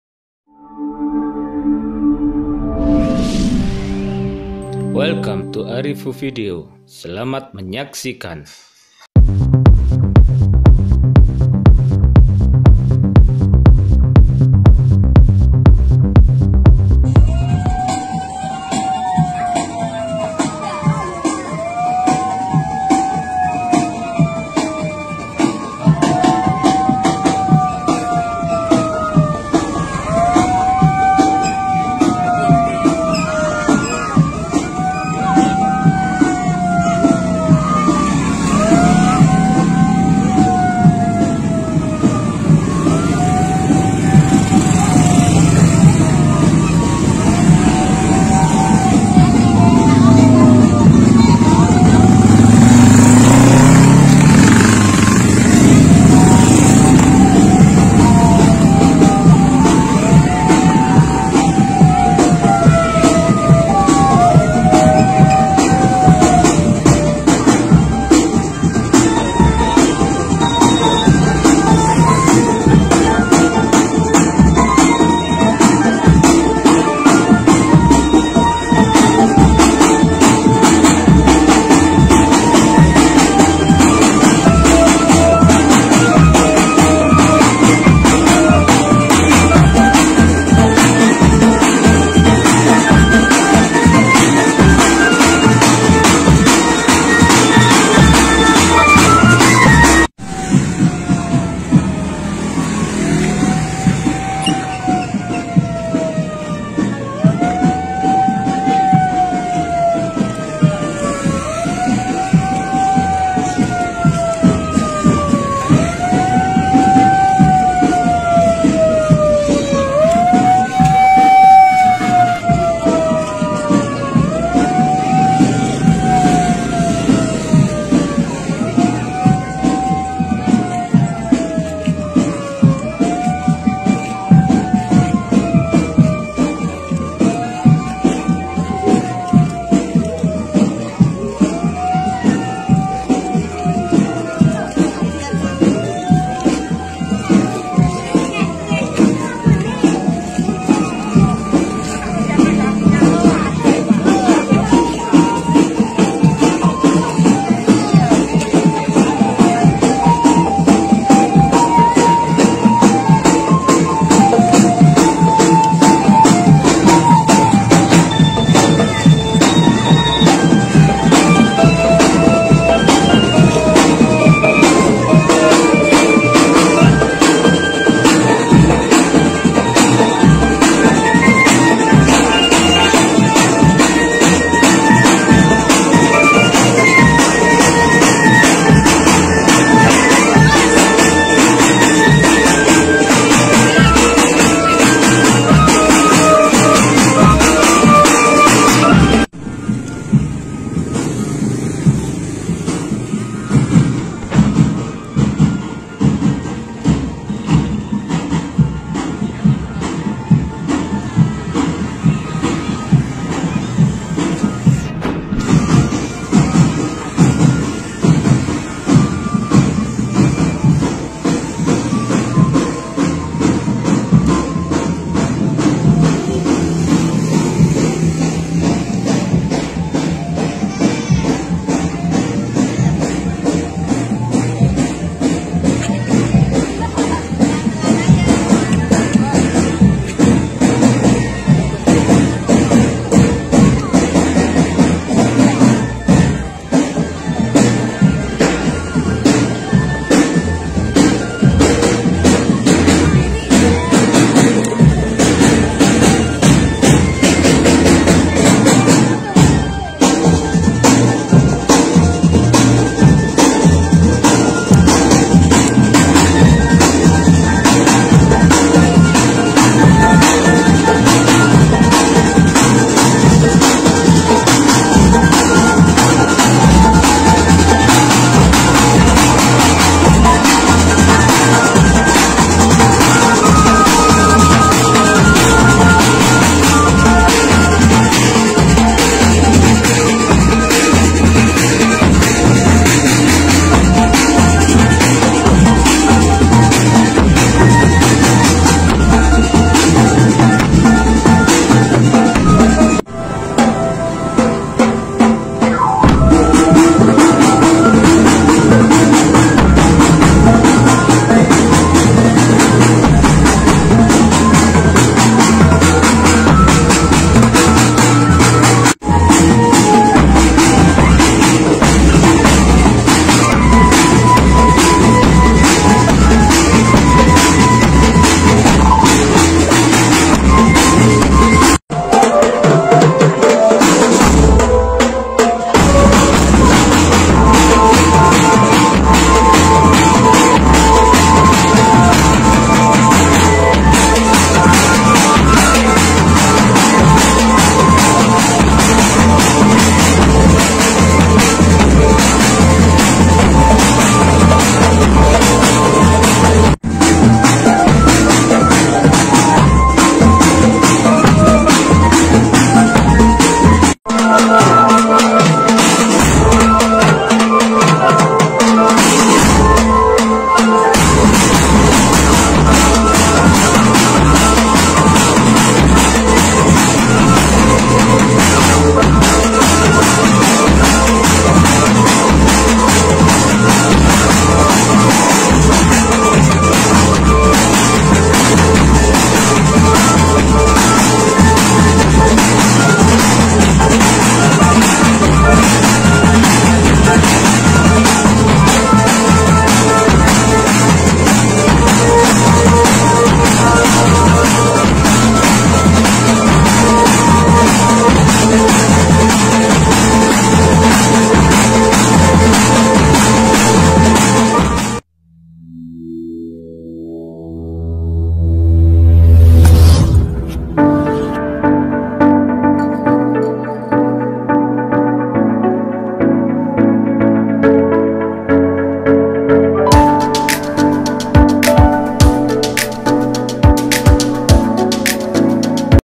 Kirab santri TPQ Nurul Iman dan Madin Darus Salam Randupitu diiringi Drum Band New Cover Plipir Atas
Dalam Rangka Haflah Akhirus Sanah santri TPQ Nurul Iman dan Madin Darus Salam Randupitu ke 20 pada tanggal 19 Maret 2020